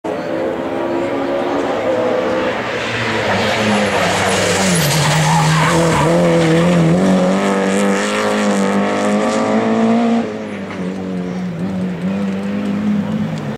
"THE FLAMING ESCORT"-Lars-Erik Björklöv Ford escort MK 2
Lars-Erik Björklöv:s häftigt lackerade ford escort  MK 2...så häftig lack att jag tycker att den borde heta" the flaming escort...eller vad tycker ni?